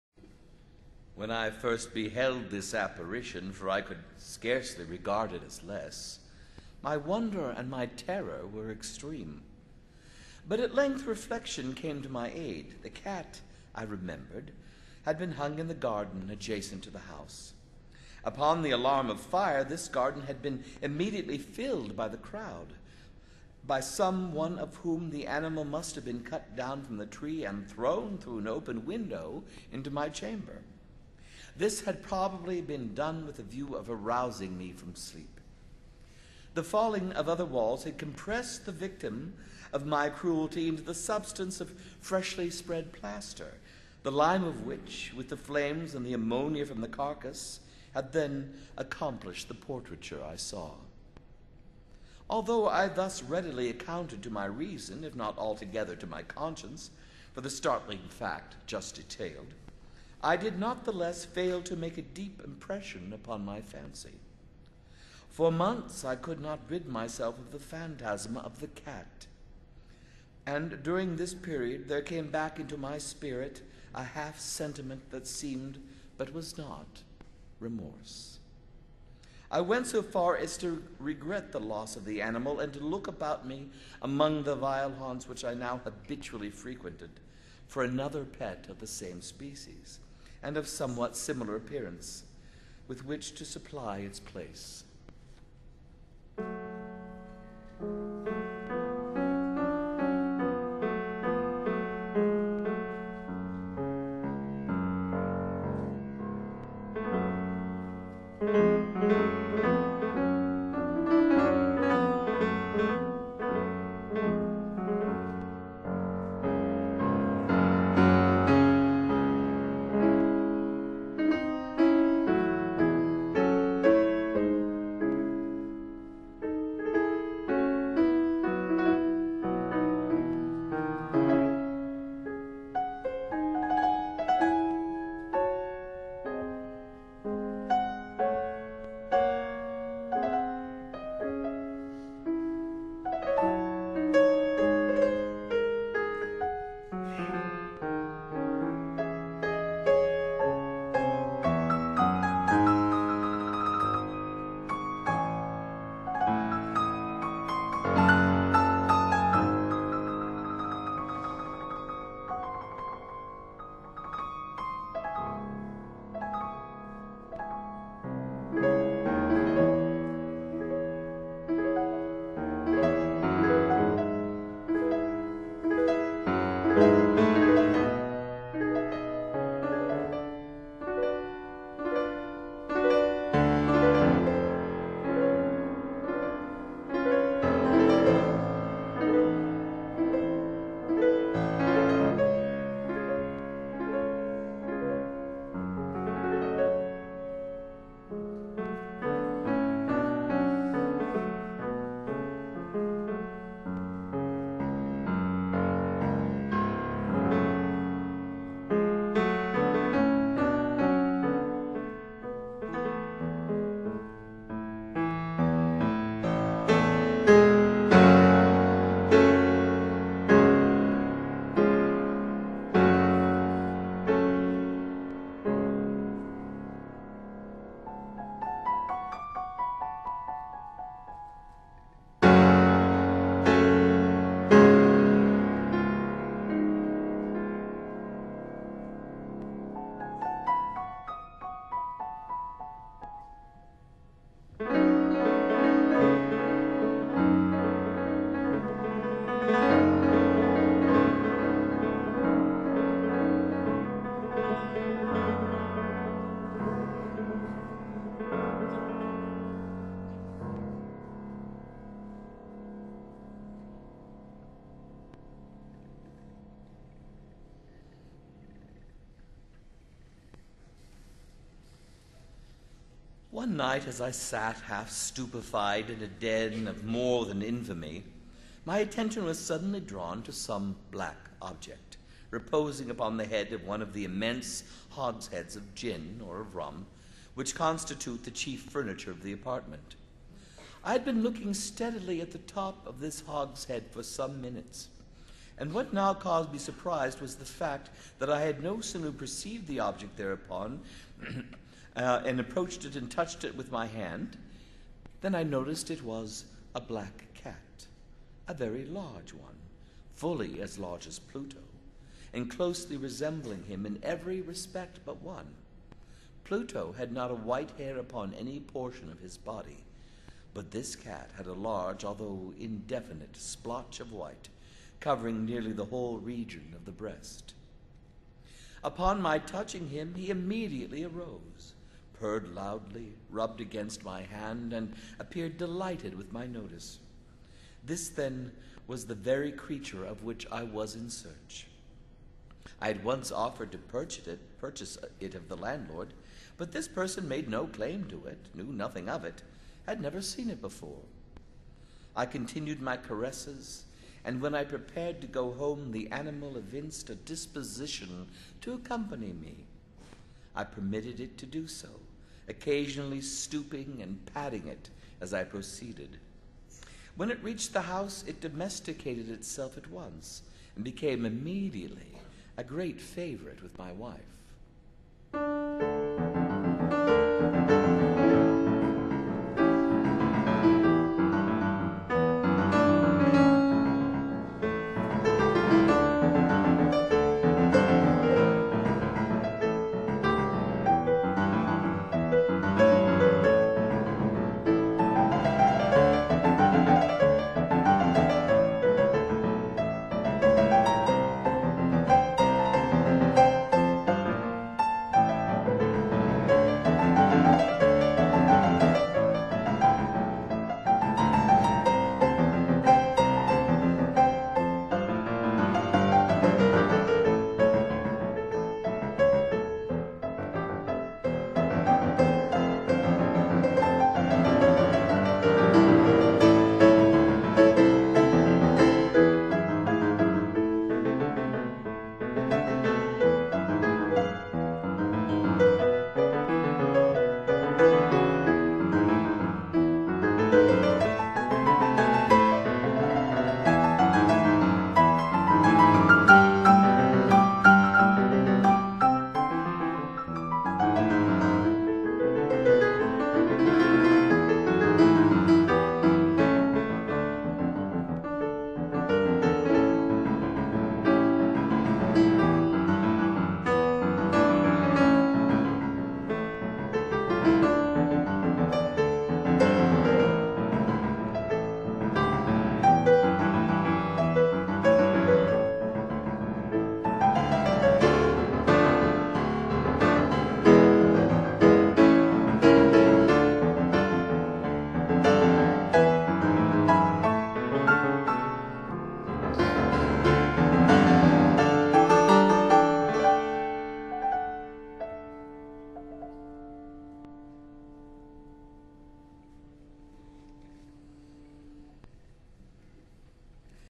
THE BLACK CAT - 3 of 6
Edgar Allan Poe's 'The Black Cat', read by Dr. David Hopes, music composed and performed by Nathan Shirley

This is the complete story, broken into sections, and each section is followed by a piano composition inspired by the story.

Video by Aaron Shirley and Thomas Fant.